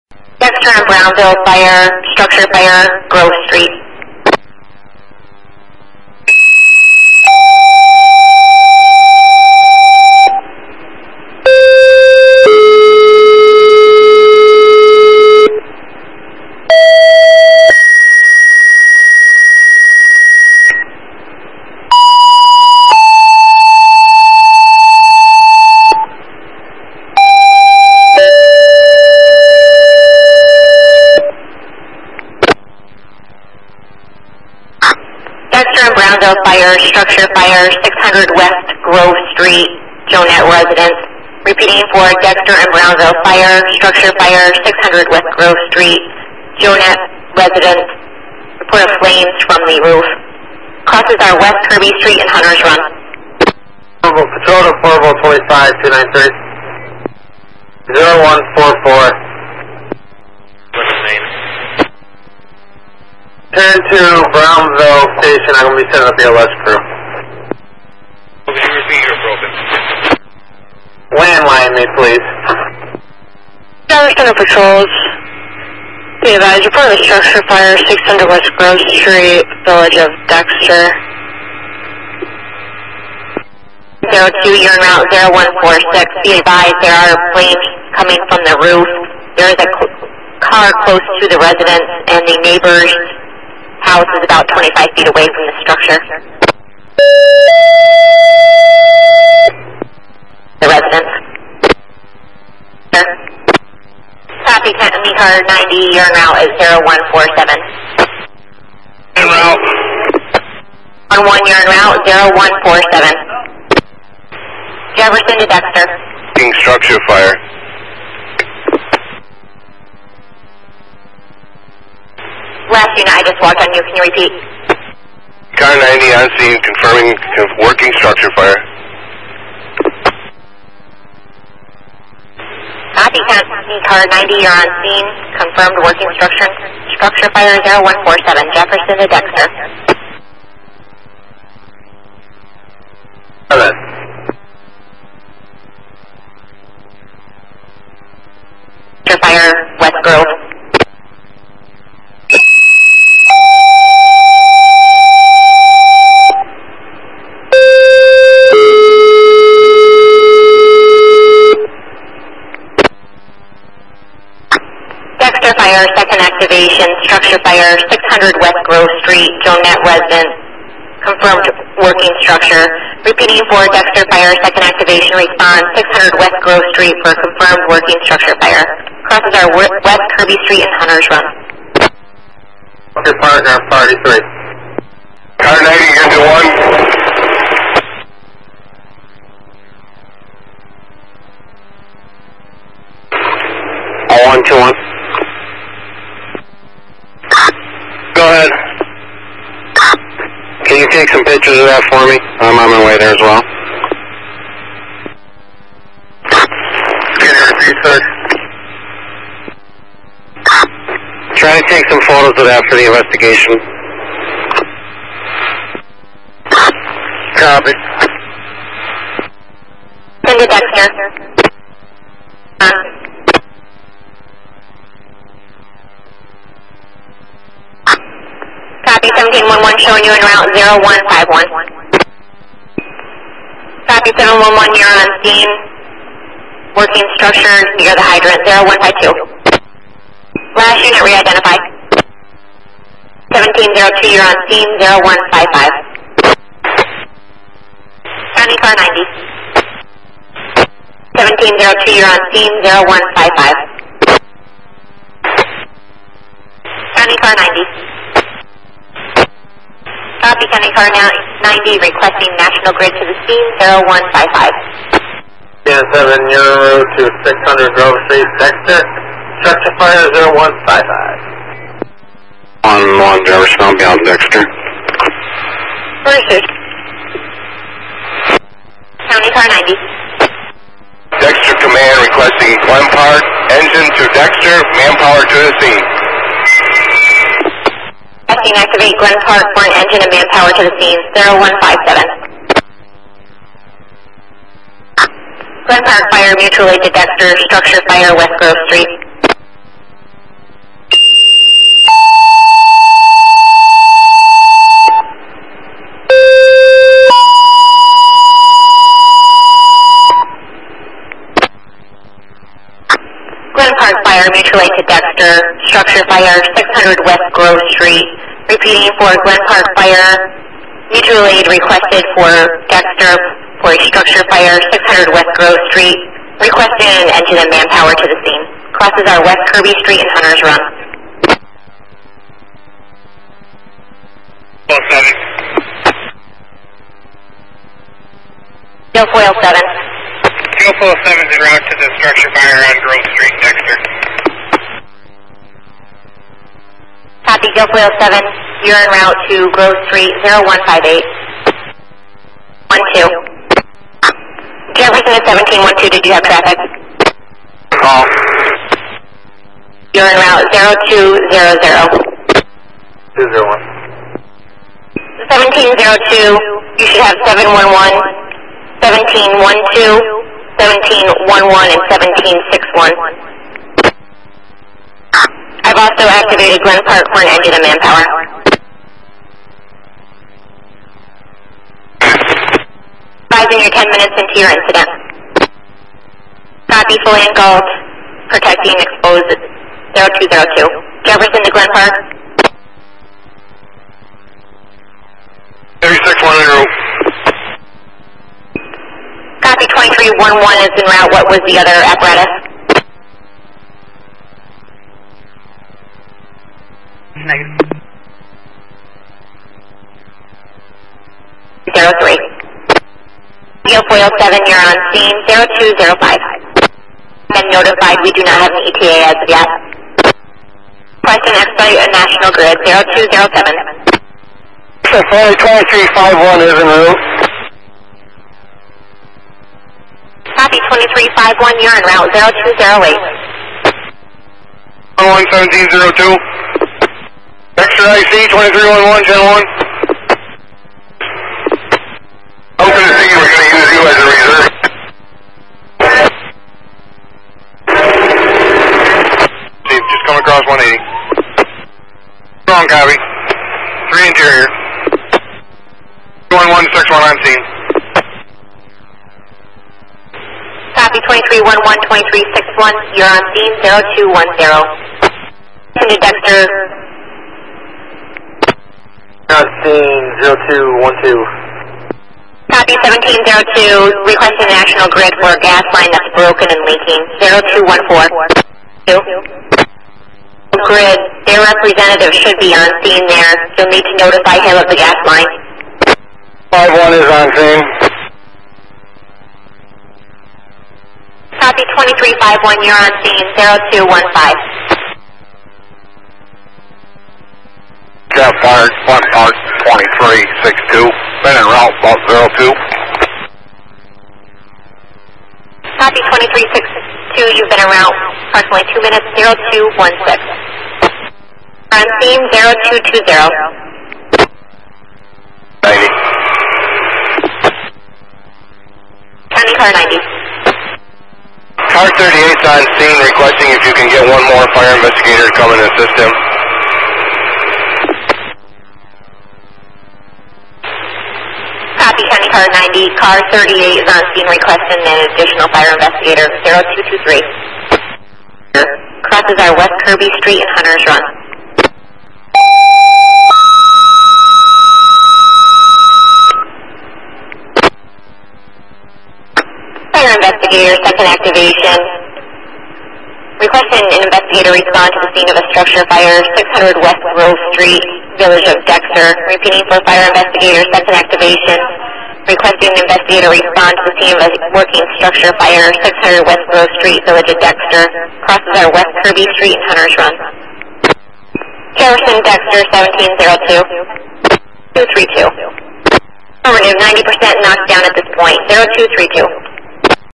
3/3/2012 West Grove St. Fire, Dexter, NY
Structure fire on West Grove St. in Dexter, NY